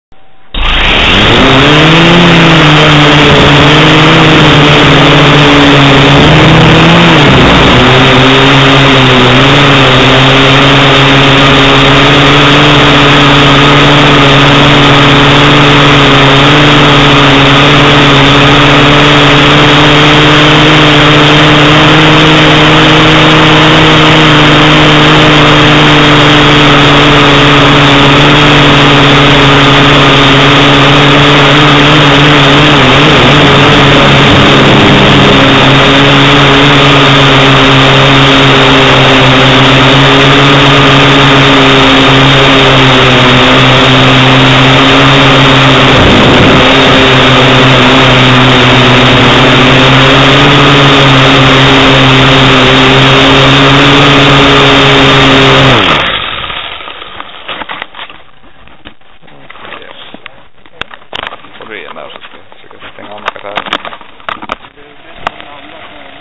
Liten flygtur med MQXn
Filmar från MQXn på Norbergs Kristiflygar-träff.
Man ser lite av allt typ :)